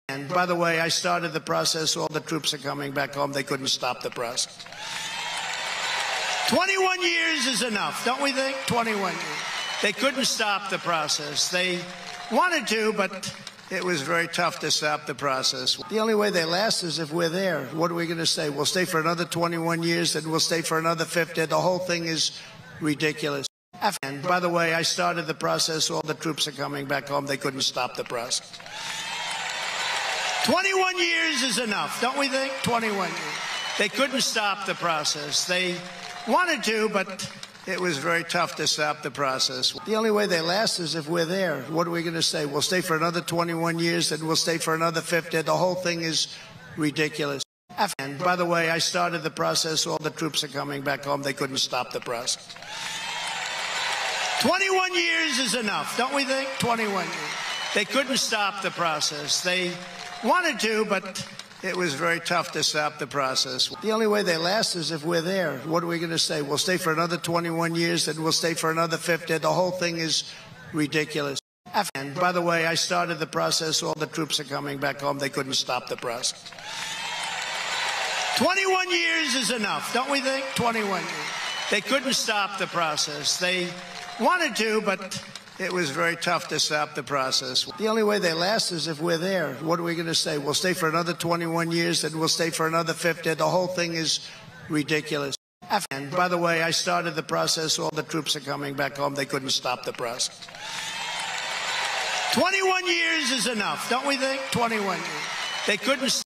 Afghanistan: aged like milk video fixed
Trump pulled out 15000 troops from Afghanistan after he released 5000 taliban fighters.

Only leaving about 2500 American troops..
Do the math.

Trump withdrew from Afghanistan last year.

If anything Biden did a great job keeping Afghanistan for so long with so few troops